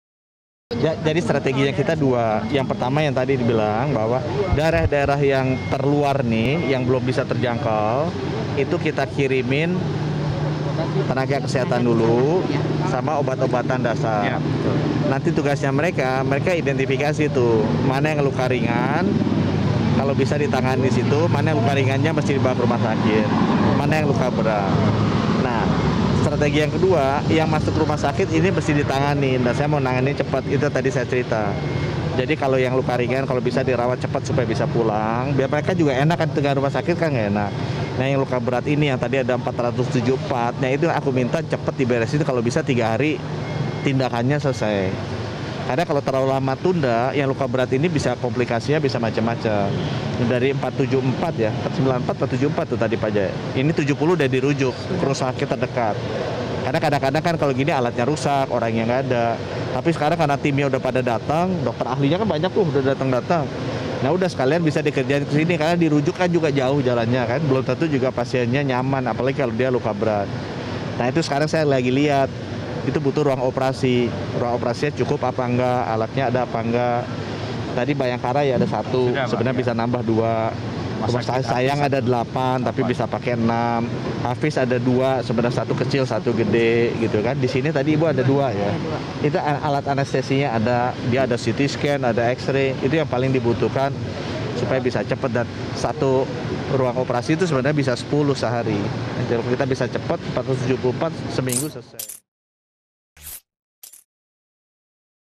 MENKES UNGKAP 2 STRATEGI PENANGANAN KORBAN GEMPA DI CIANJUR
Menteri Kesehatan (Menkes) RI, Budi Gunadi Sadikin mengungkapkan 2 strategi untuk penanganan korban gempa Cianjur. Pertama, tenaga medis dan obat-obatan dasar dikirim ke daerah yang sulit terjangkau dan melakukan seleksi korban gempa Cianjur yang nantinya akan segera dibawa ke rumah sakit. Kedua, korban gempa Cianjur di rumah sakit harus cepat ditangani.